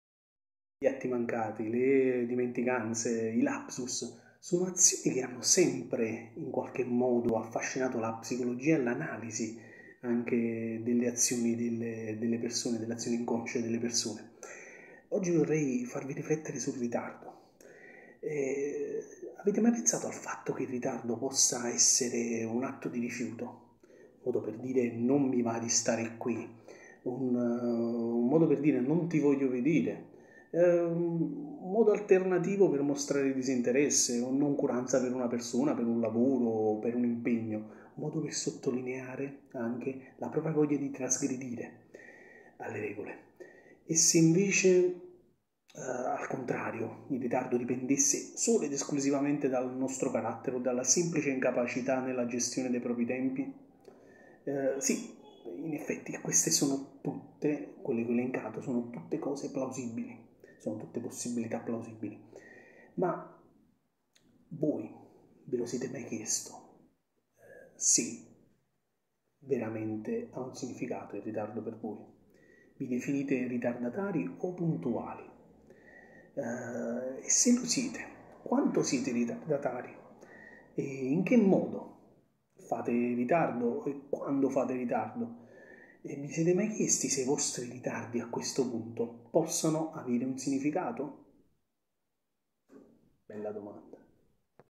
IL SIGNIFICATO PSICOLOGICO DEL RITARDO
Vi siete mai chiesti se i vostri ritardi possano avere un significato?
Un atto di rifiuto? Un modo per dire: “non mi va di stare qui” ; “non ti voglio vedere” ?
Insomma, un modo alternativo per mostrare disinteresse..
Quindi dobbiamo considerare il "Ritardo" come atto con un significato sottostante o come semplice comportamento privo di un reale significato?